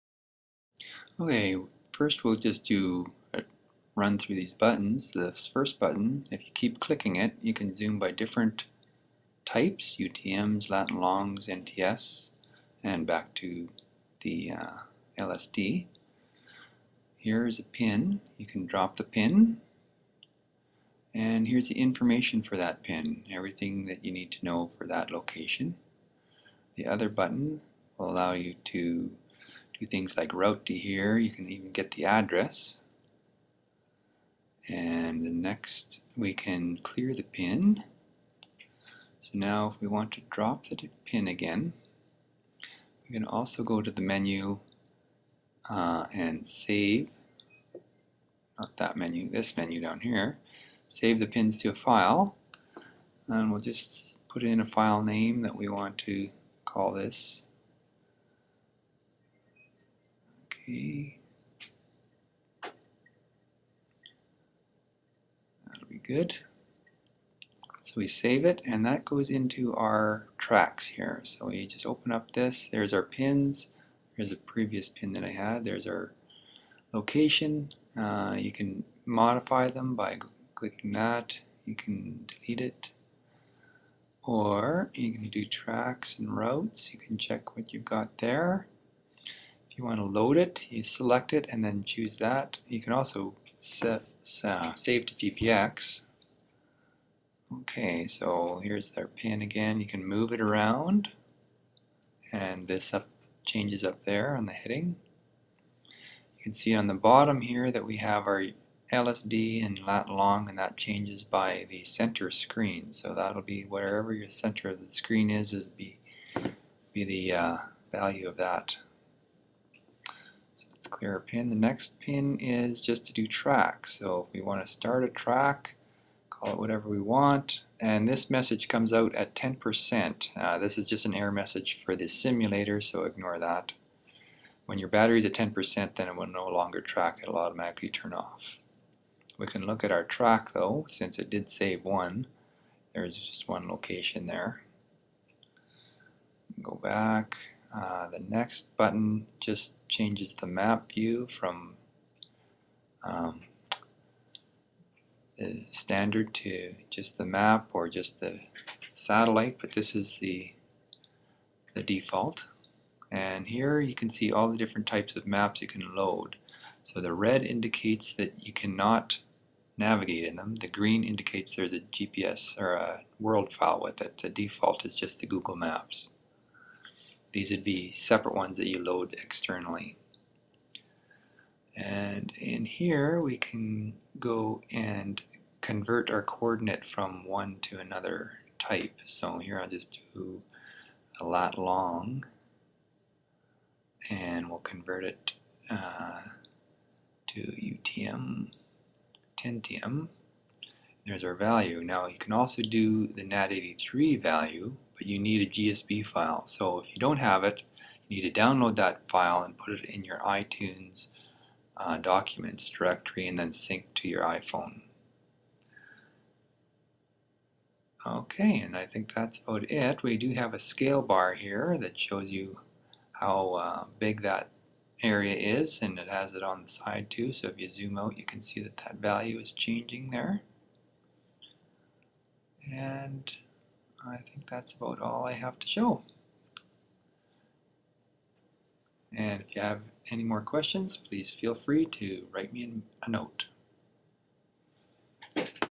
LSD Nav Pro Quick Overview
Gives a quick overview of the features currently available in the LSD Nav Pro app.